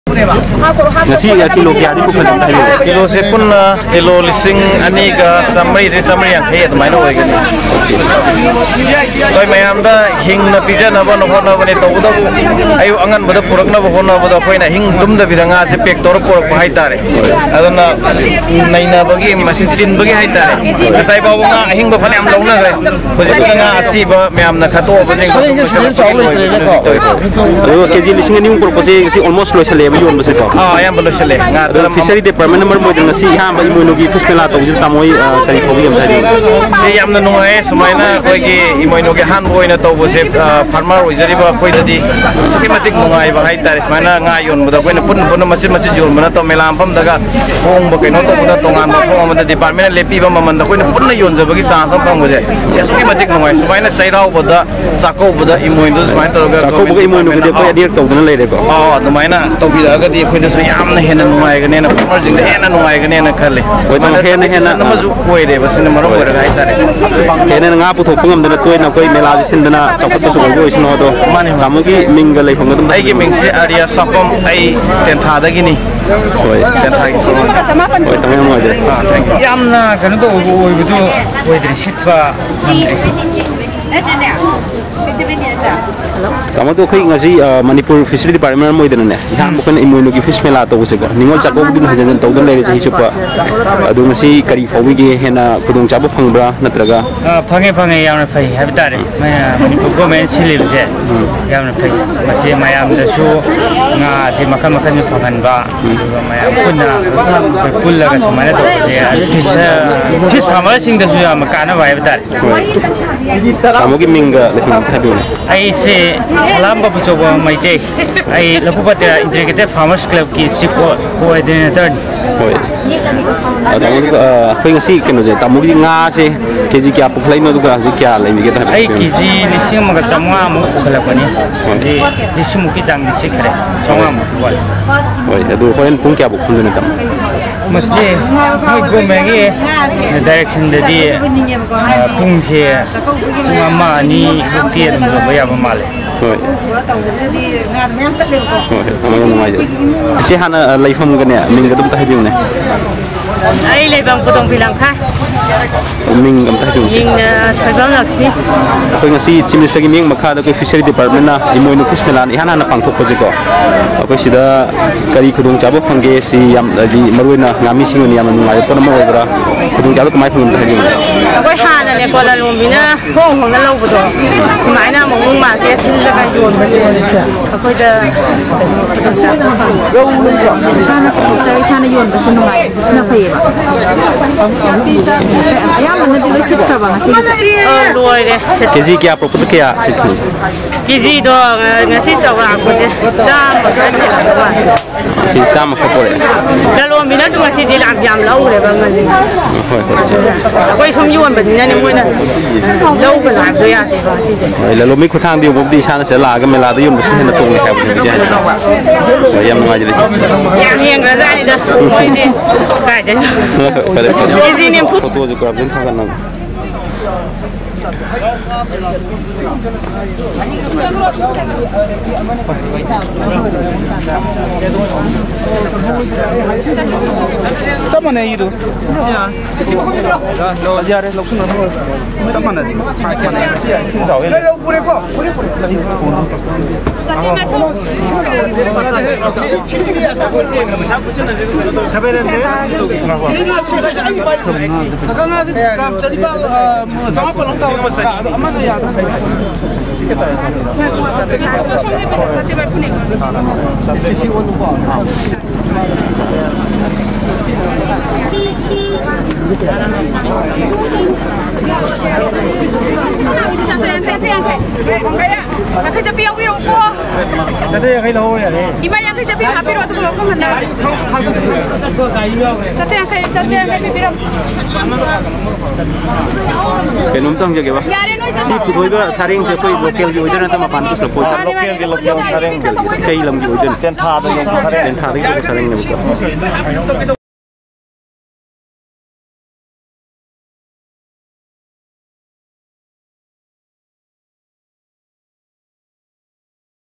A Glimpse of the First Emoinu Fish Fair at Hapta Kangjeibung;
Hundreds Throng into Buy Fish at First Emoinu Fish Fair
Hundreds of families thronged in today at the First Emoinu Fish Fair at Hapta Kangjeibung to buy fish for the Emoinu Eratpa.